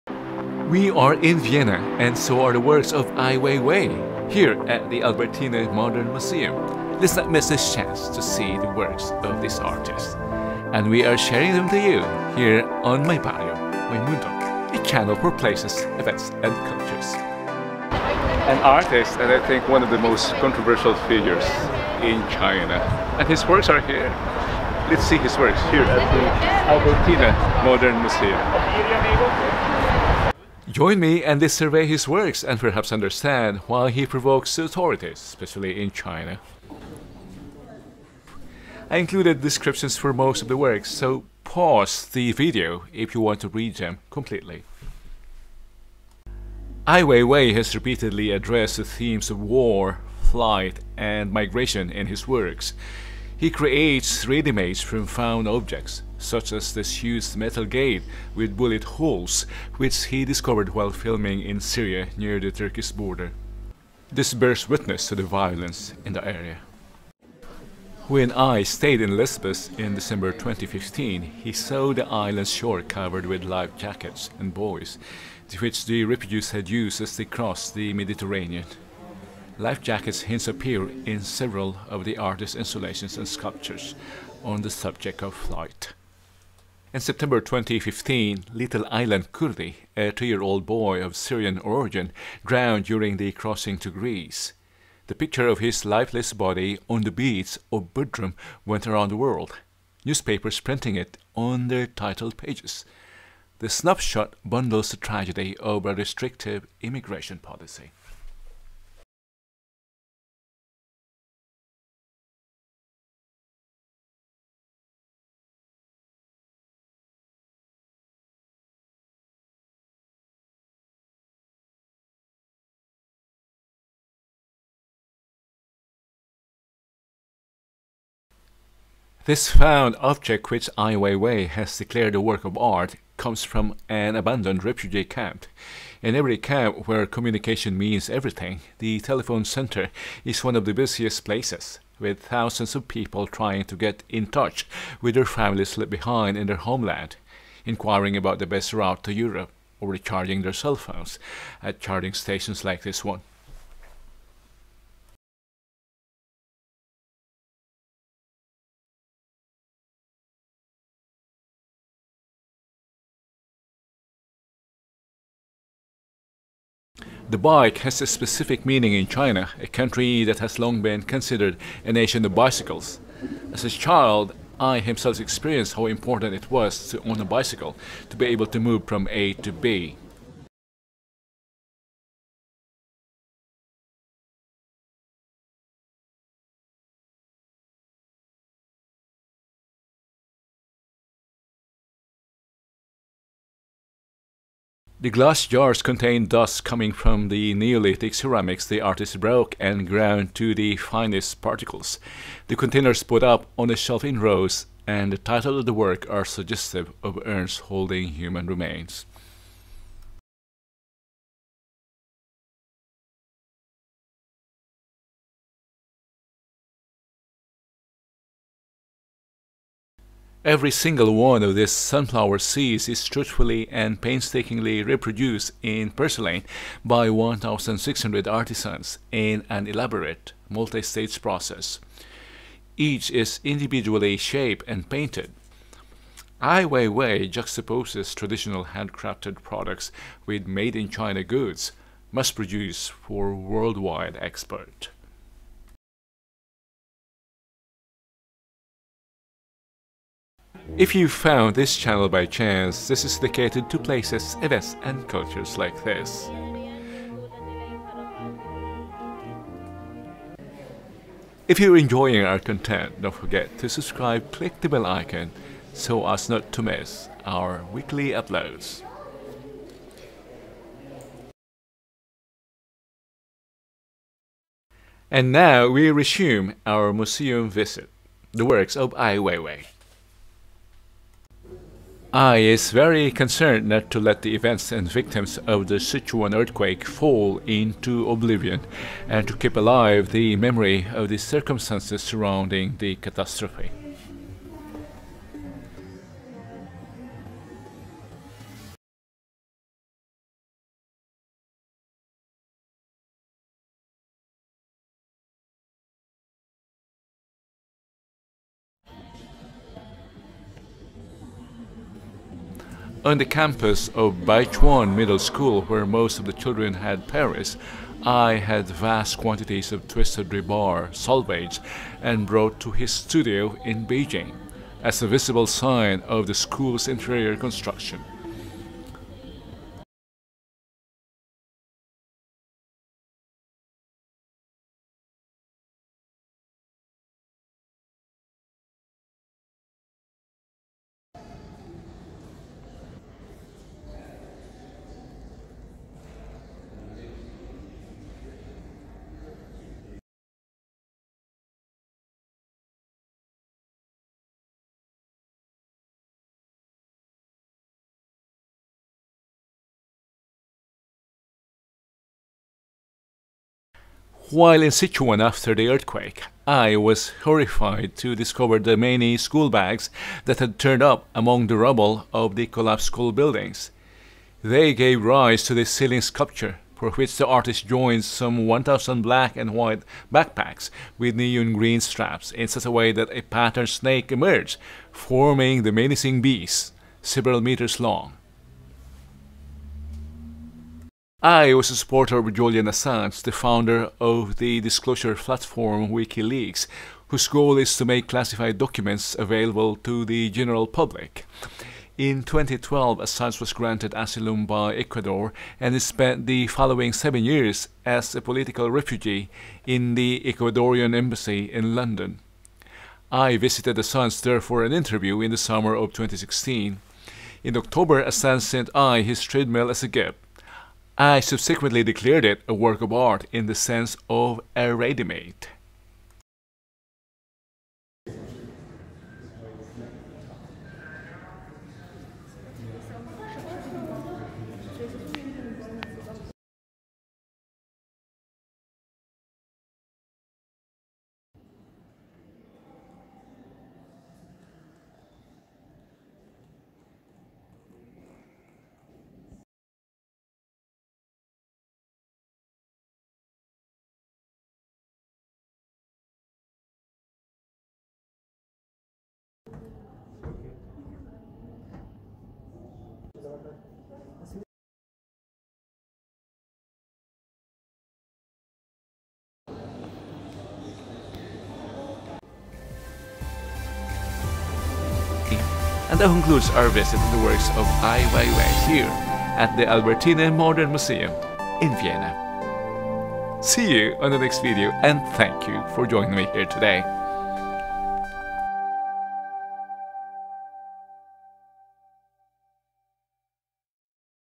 Works of Ai Wei Wei at the Albertina Modern in Vienna - a museum visit (4k UHD) (2022)
We are in Vienna and so are the works of Ai Wei Wei. Join me here at the Albertina Modern and let us see his works, and perhaps understand why he is controversial specially in China.

The Albertina modern dedicates itself to the impressive oeuvre of Ai Weiwei in a moving spring exhibition. It is the most comprehensive retrospective of the world-famous Chinese conceptual artist and activist ever curated.

Bicycles, letterboxes, shoes, violins, thousands of sunflower seeds, and a treadmill – objects from the four decades of artistic work by Ai Weiwei. They all represent topics that are close to the heart of the untiring activist and critic of authoritarian systems. They can be seen in the form of numerous installations in the new exhibition "Ai Weiwei. In Search of Humanity" at the Albertina modern.

The museum in the Künstlerhaus dedicates its most comprehensive retrospective ever to the Chinese conceptual artist in spring 2022. Numerous ready-mades, wall works, sculptures, photographs, and films give an overview of his oeuvre. Ai Weiwei is considered to be one of the most important artists of our time, who always focuses his gaze wherever he sees freedom of opinion and human rights in danger.